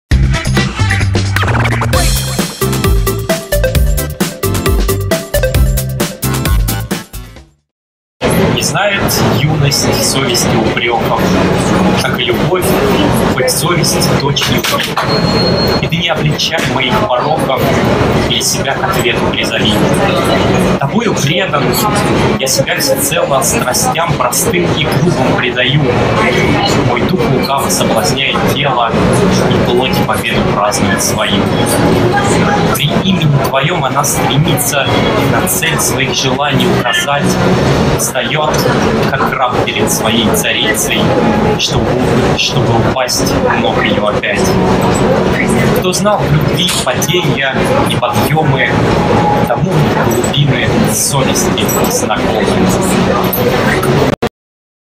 Владислав Носивской Гимназия №82 Краснодар
Чемпионат по чтению вслух "Страница 17", Краснодар, 2017 г.